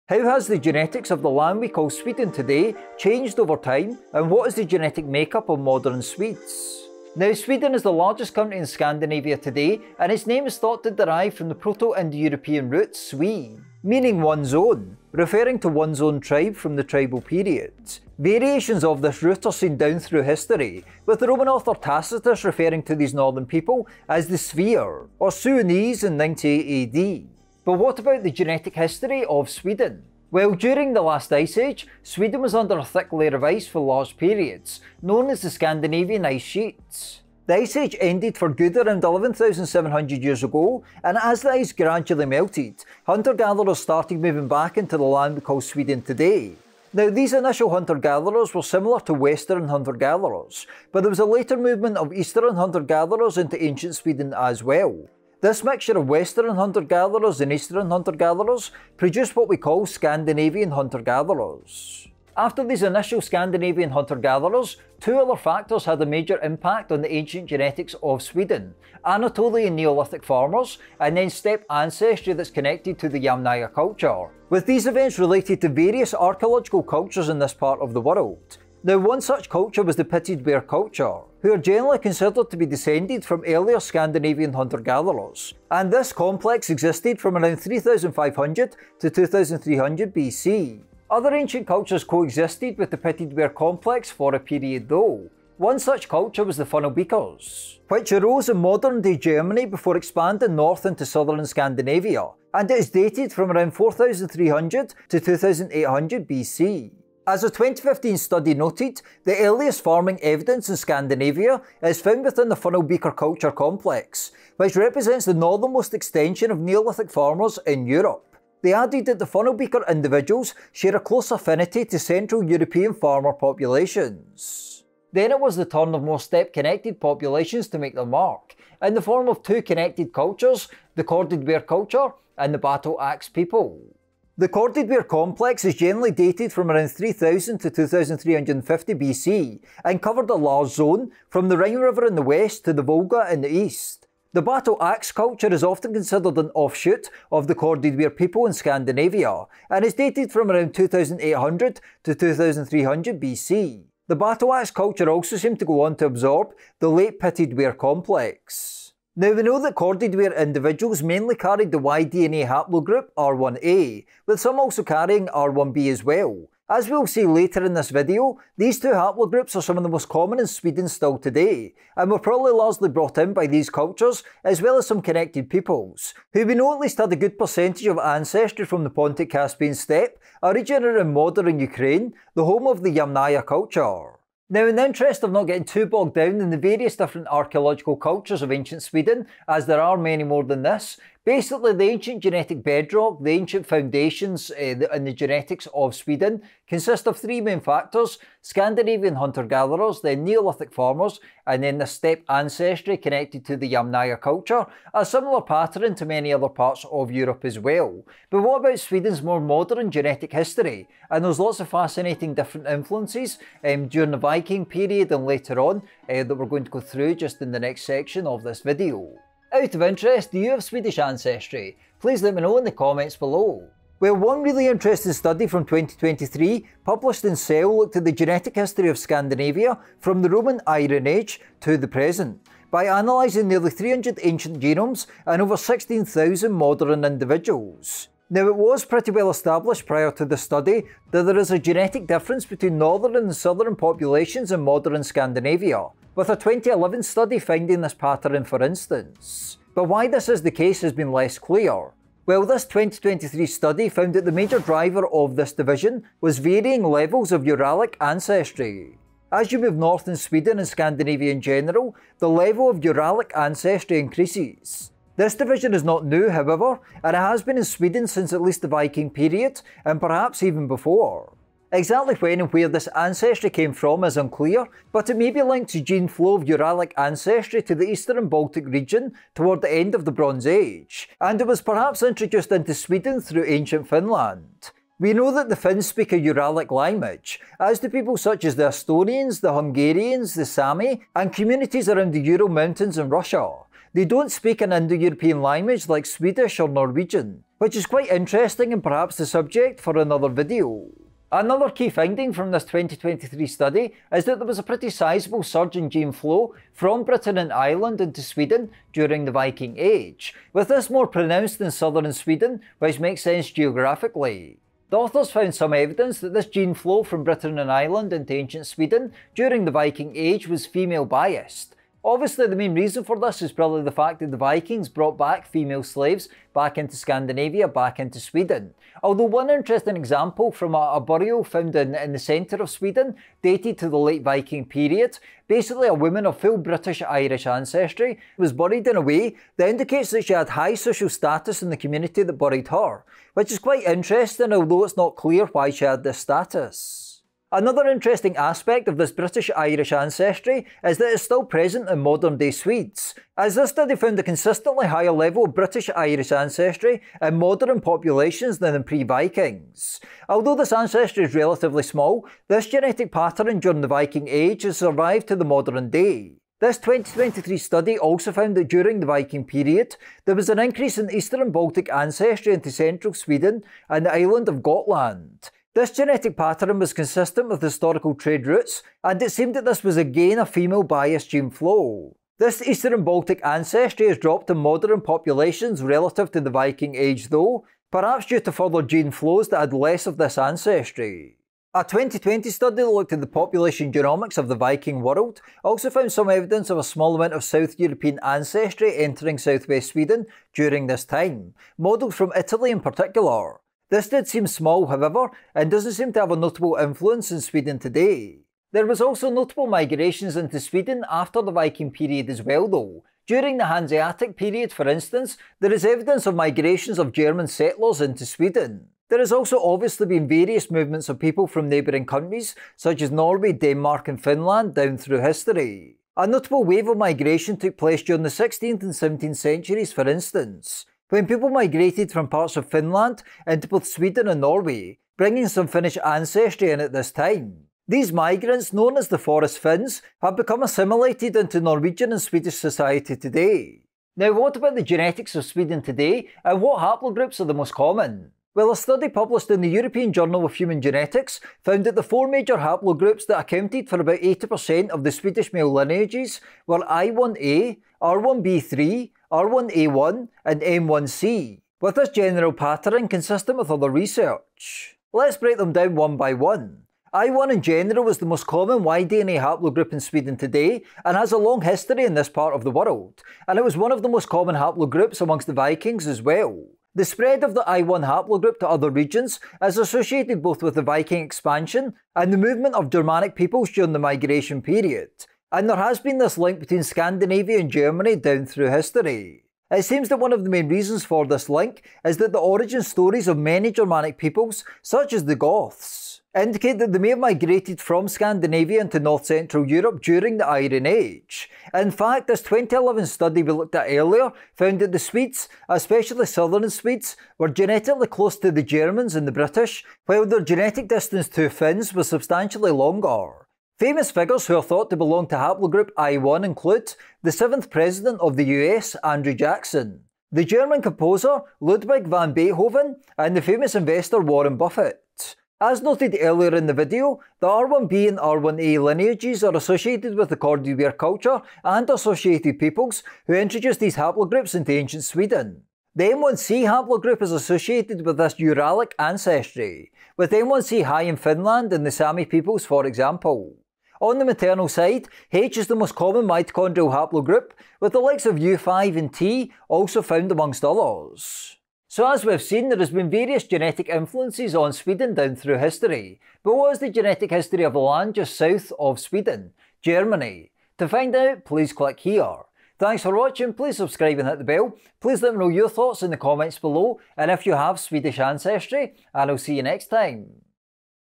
Swedish DNA: What is the Genetic History of Sweden?
Chapters:
0:00 Swedish DNA
0:30 Early Genetic History and Ancient Cultures
3:43 Viking Period
6:48 Post-Viking Period
7:24 Modern Genetics of Sweden & Haplogroups

How has the genetics of the land we call Sweden today changed over time and what is the genetic makeup of modern Swedes? Now Sweden is the largest country in Scandinavia and its name is thought to derive from the Proto-Indo-European root *s(w)e, meaning "one's own", referring to one's own tribe from the tribal period. Variations of this root are seen down through history, with the Roman author Tacitus referring to these northern people as the Svear, or Suiones, in 98 AD.

But what about the genetic history of Sweden? Well during the last ice age, Sweden was under a thick sheet of ice for large periods, known as the Scandinavian Ice Sheet. The ice age ended for good around 11,700 years ago, and as the ice gradually melted, hunter gatherers started moving back into the land we call Sweden today. Now these initial hunter gatherers were similar to Western Hunter Gatherers, but there was a later movement of Eastern Hunter Gatherers into ancient Sweden as well. This mixture of Western Hunter Gatherers and Eastern Hunter Gatherers produced what we call Scandinavian hunter-gatherers.

Sources:

Rodríguez-Varela, R. The genetic history of Scandinavia from the Roman Iron Age to the present 2023, Cell 186, 32–46 The genetic history of Scandinavia from the Roman Iron Age to the present: Cell

Salmela E, Lappalainen T, Liu J, Sistonen P, Andersen PM, Schreiber S, et al. (2011) Swedish Population Substructure Revealed by Genome-Wide Single Nucleotide Polymorphism Data. PLoS ONE 6(2): e16747.

DNA studies give a new view of Sweden’s history - Stockholm University (su.se)

Surprised to see Sweden & Denmark or Norway in your ethnicity results? (ancestry.com.au)

Northern and southern Swedes are genetically different | Karolinska Institutet (ki.se)

Karlsson, A., Wallerström, T., Götherström, A. et al. Y-chromosome diversity in Sweden – A long-time perspective. Eur J Hum Genet 14, 963–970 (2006).

Humphreys K, Grankvist A, Leu M, Hall P, Liu J, Ripatti S, et al. (2011) The Genetic Structure of the Swedish Population. PLoS ONE 6(8): e22547.

Haplogroup I1 (Y-DNA) - Eupedia

Scandinavian Ice Sheet | History, Geography, Map, & Definition | Britannica

Haplogroup N1c (Y-chromosomal DNA) - Eupedia

Malmström Helena et al. 2015 Ancient mitochondrial DNA from the northern fringe of the Neolithic farming expansion in Europe sheds light on the dispersion process Phil. Trans. R. Soc. B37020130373

Malone, Kemp. “The Suiones of Tacitus.” The American Journal of Philology, vol. 46, no. 2, 1925, pp. 170–76. JSTOR, Accessed 21 Aug. 2024.

Margaryan, A., Lawson, D.J., Sikora, M. et al. Population genomics of the Viking world. Nature 585, 390–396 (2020).

Haplogroup H (mtDNA) - Eupedia

Western hunter-gatherer - Wikipedia

Eastern hunter-gatherer - Wikipedia

Scandinavian hunter-gatherer - Wikipedia

Funnelbeaker culture - Wikipedia

History and genetics of the Corded Ware culture - Eupedia

Battle Axe culture - Wikipedia

Hanseatic League - Wikipedia

Forest Finns - Wikipedia